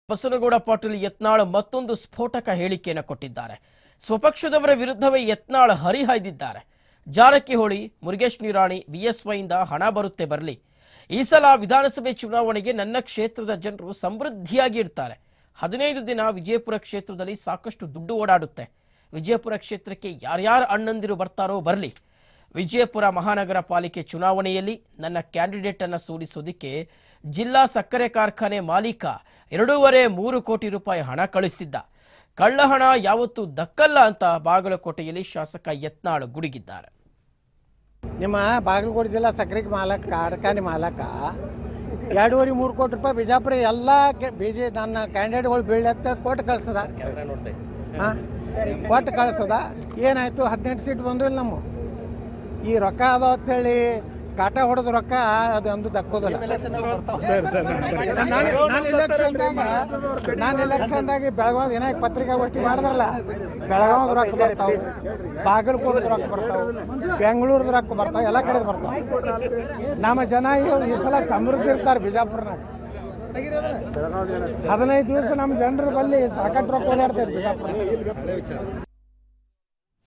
ಚುನಾವಣೆ ಹೊತ್ತಲ್ಲಿ ಶಾಸಕ ಬಸನಗೌಡ ಪಾಟೀಲ್ ಯತ್ನಾಳ್ ಮತ್ತೊಂದು ಸ್ಫೋಟಕ ಹೇಳಿಕೆ!
Btv News Kannada : ಚುನಾವಣೆ ಹೊತ್ತಲ್ಲಿ ಶಾಸಕ ಬಸನಗೌಡ ಪಾಟೀಲ್ ಯತ್ನಾಳ್ ಮತ್ತೊಂದು ಸ್ಫೋಟಕ ಹೇಳಿಕೆ..  ಸ್ವಪಕ್ಷದವ್ರ ವಿರುದ್ಧವೇ ಹರಿಹಾಯ್ದ ಯತ್ನಾಳ್!